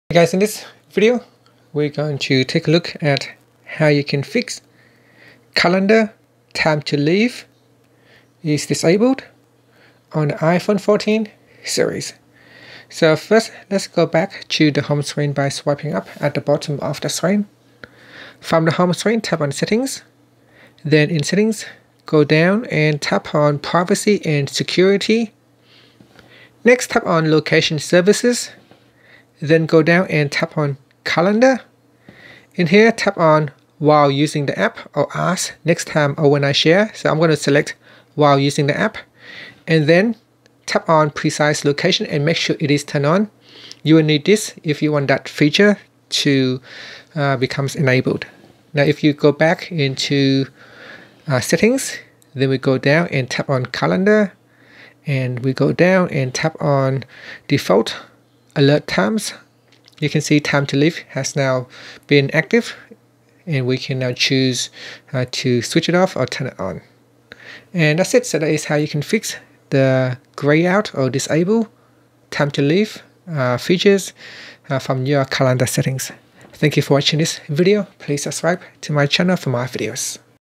iPhone 14's/14 Pro Max: How to Fix Calendar 'Time to Leave' Disabled / Grayed Out
Learn how you can fix the calendar 'Time to Leave' that is disabled or grayed out on the iPhone 14/iPhone 14 Pro Max/iPhone 14 Plus.

This video demonstration is done on iOS 16.